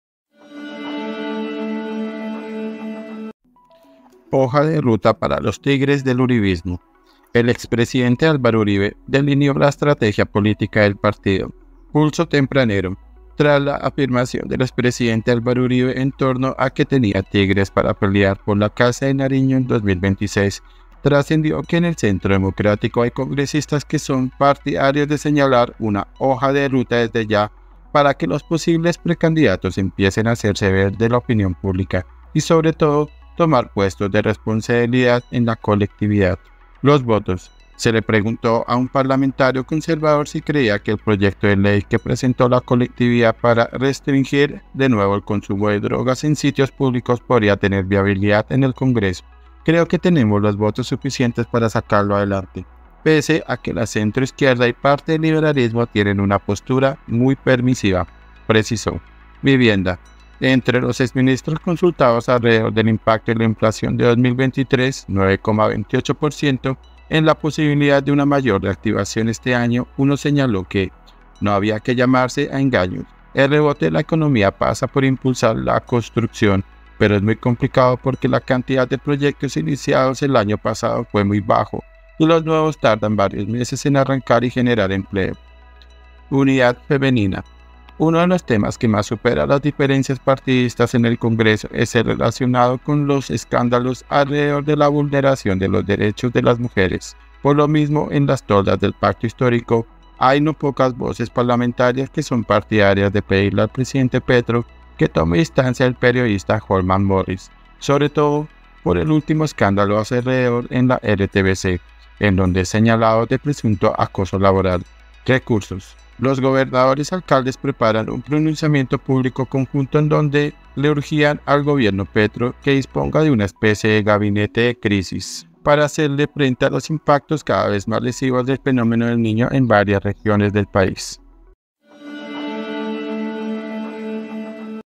⭕ HOJA DE RUTA PARA LOS "TIGRES" DEL URIBISMO
PULSO TEMPRANERO.  Tras la afirmación del expresidente Álvaro Uribe en torno a que tenía “tigres” para pelear por la Casa de Nariño en 2026, trascendió que en el Centro Democrático hay congresistas que son partidarios de señalar una hoja de ruta desde ya para que los posibles precandidatos empiecen a hacerse ver de la opinión pública y, sobre todo, tomar puestos de responsabilidad en la colectividad.